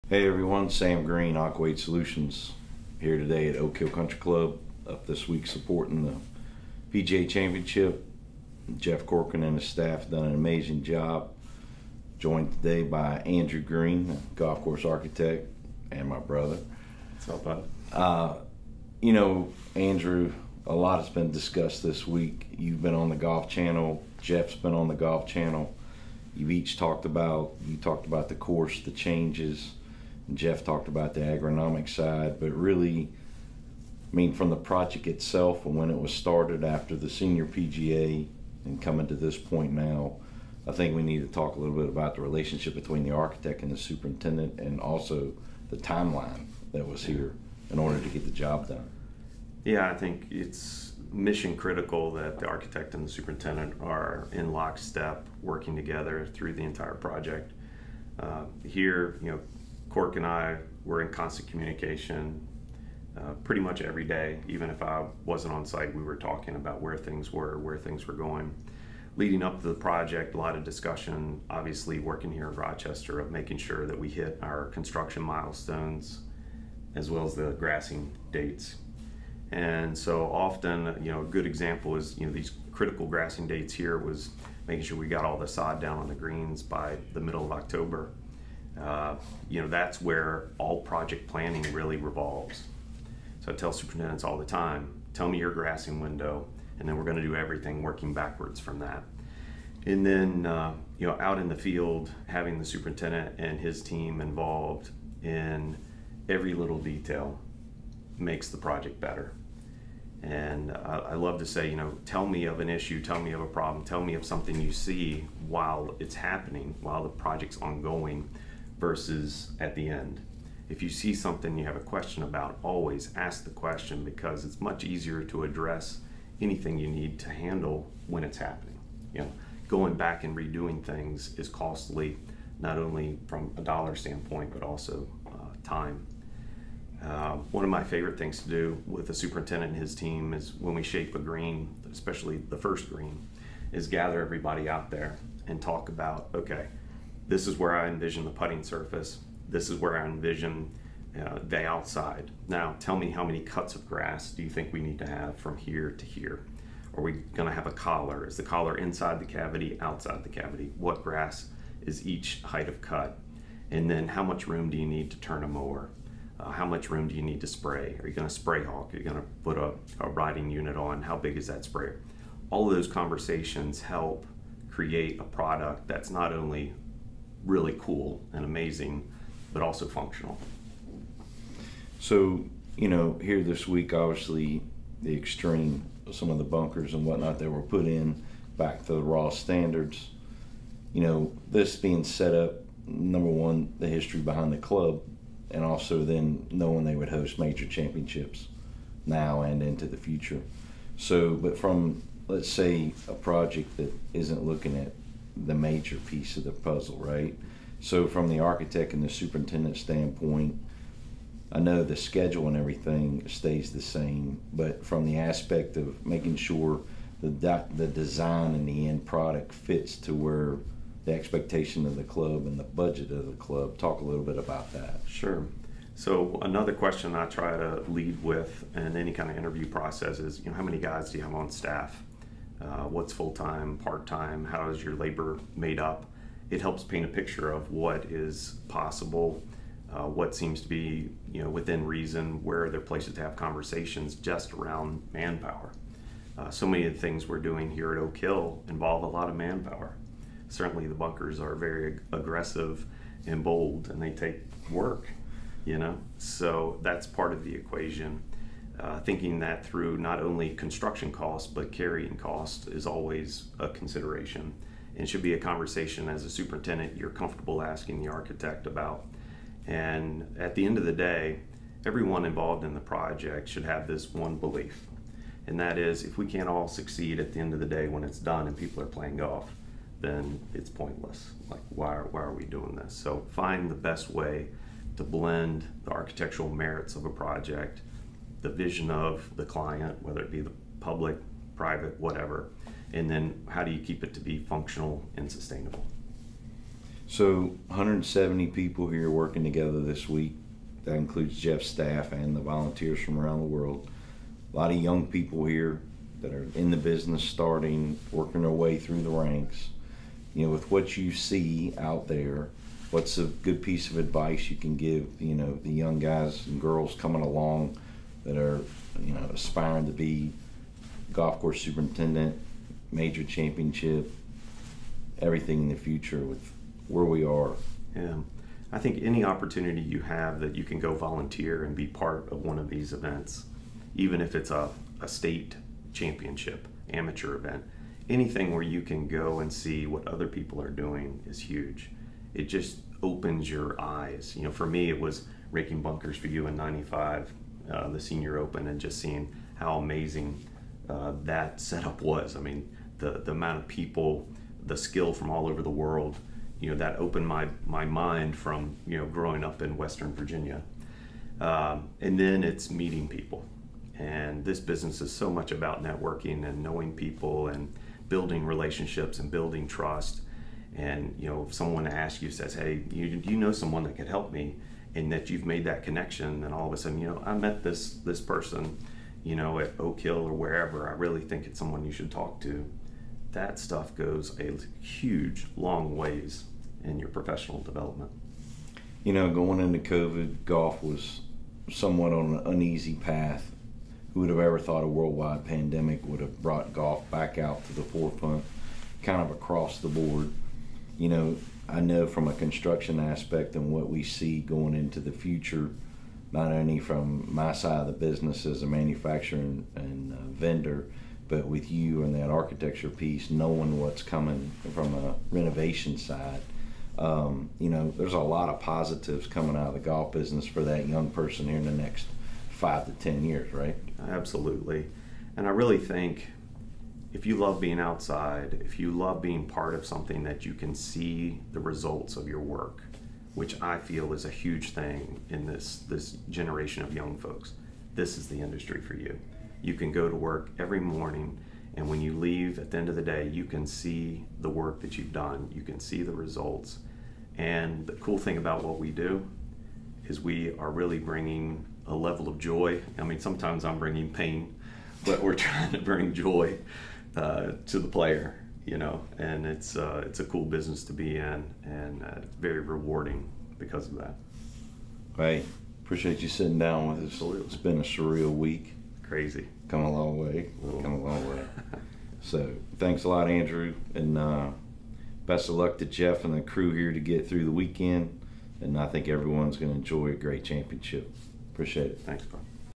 The Golf Course Architect and Golf Course Superintendent Relationship
Sam Green, President of AQUA AID Solutions sits down with Andrew Green, Golf Course Architect to discuss the importance of the relationship between the Architect and the Superintendent. "It is mission critical the architect and superintendent are in lockstep."